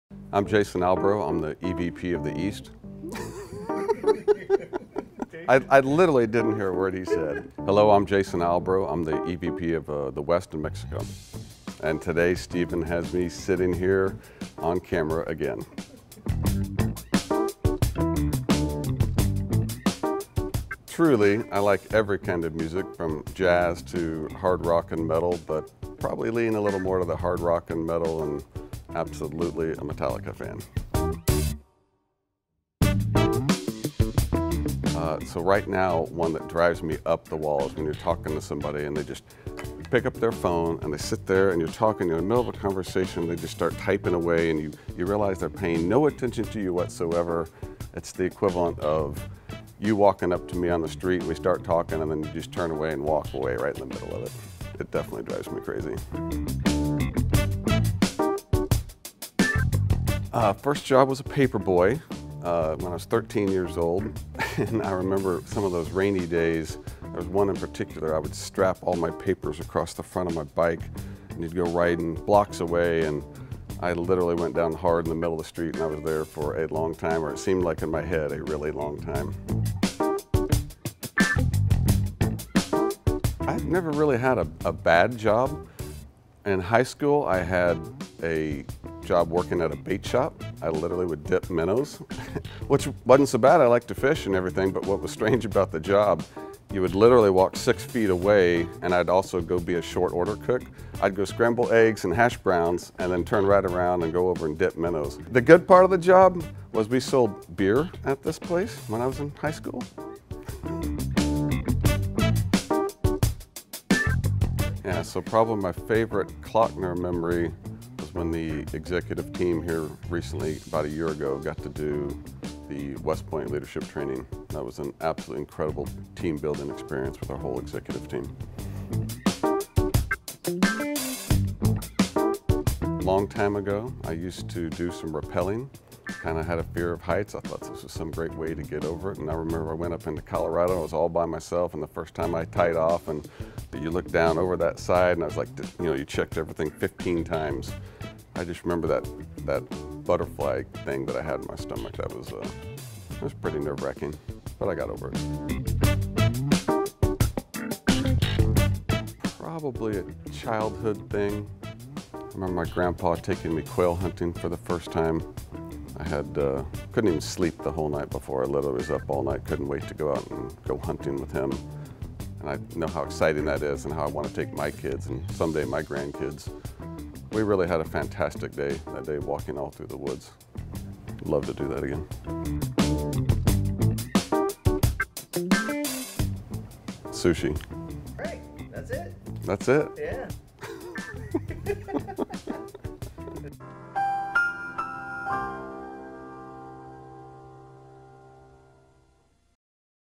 Jason Albro | The More You Know | Kloeckner Metals
Jason Albro, our EVP, West Region, on the hot seat! Watch the video to find out Jason's favorite type of music, his fondest memory at Kloeckner Metals, the scariest thing he has ever done, and much more!